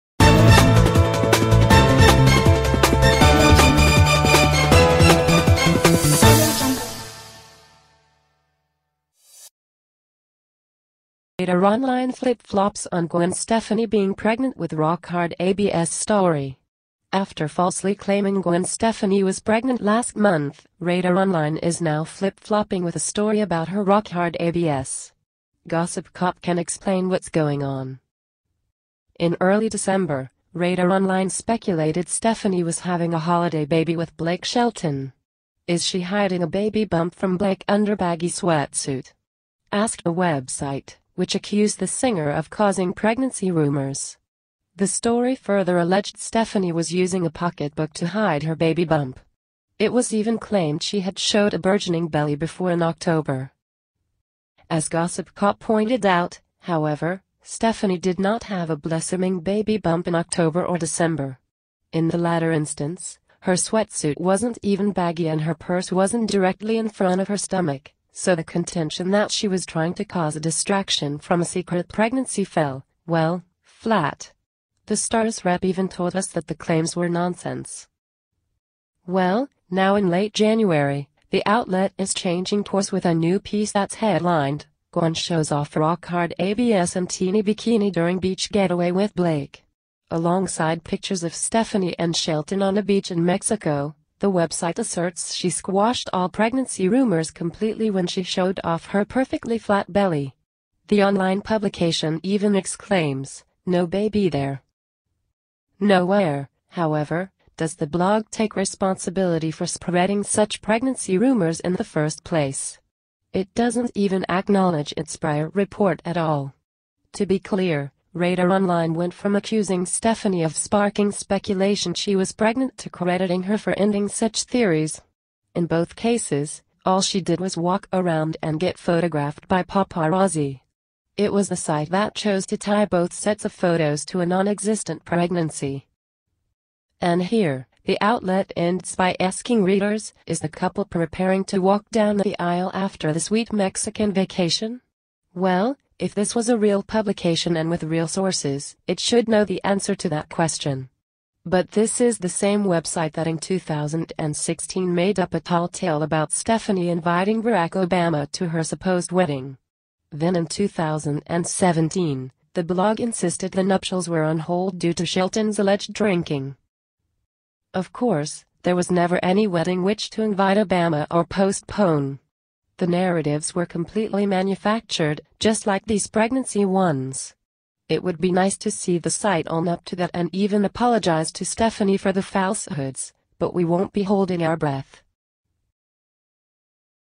RadarOnline is currently reversing the story after claiming Gwen Stefani was pregnant
After falsely claiming Gwen Stefani was pregnant last month, RadarOnline is now flip-flopping with a story about her “rock-hard abs.”